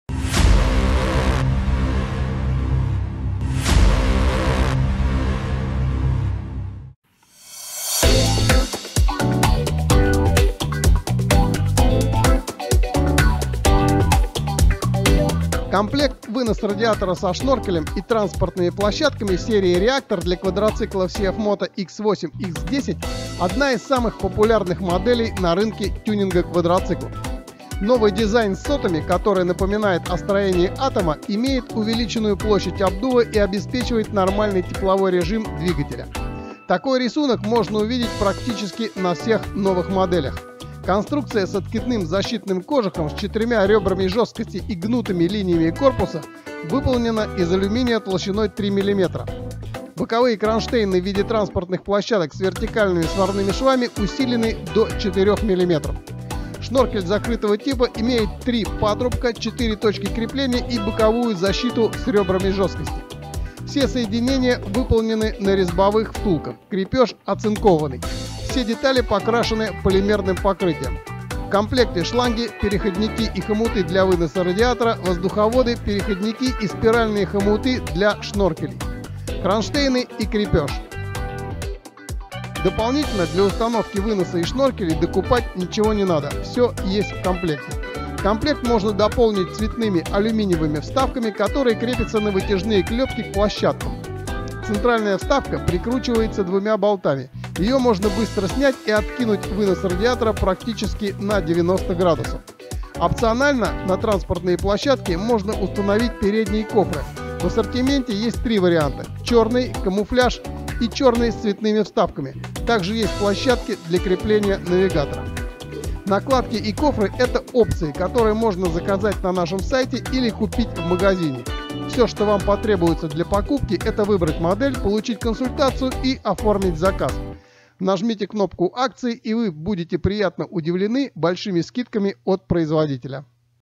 Вынос радиатора на CFMOTO X8 H.O , X10 H.O. Серия Реактор
Комплект вынос радиатора со шноркелем и транспортными площадками серии Реактор для квадроциклов CFMOTO х8-х10.  Лучшие решения по креплениям, жесткости, кронштейнам и дизайну воплощены в этой модели. Ставшая бестселлером стильная и функциональная модель с новым рисунком и логотипом может комплектоваться оригинальными кофрами и вставками разных цветов. Если Вам нужна подробная информация по комплекту, то вот ссылка на раздел официального сайта. Продукция всегда в наличии.